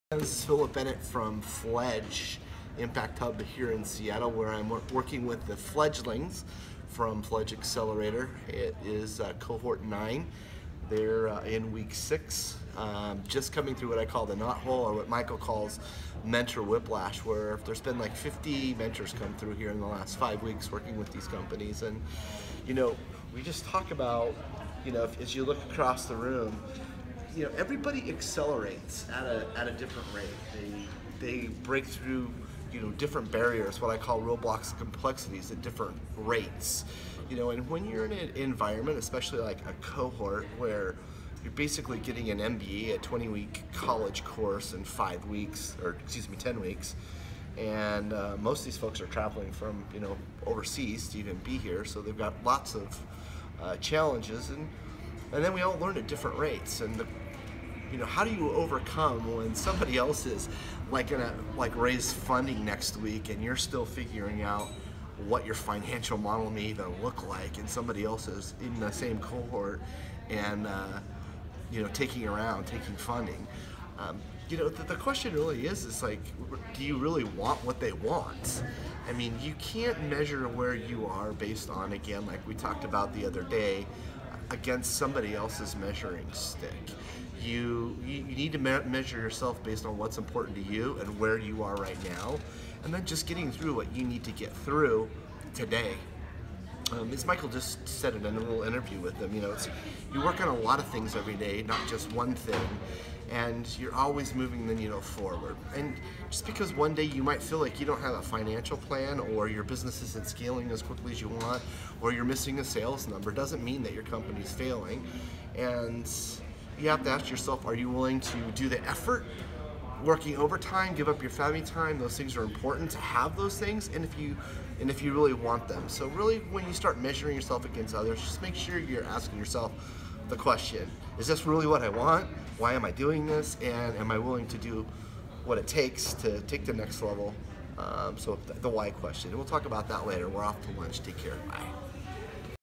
Philip D. Bennett: Do I Really Want and Will I Do What's Required?
We all have different goals and challenges in life and we strive to meet them at our own pace, in our own way. Don’tmeasure your status and progress based on others’ metrics.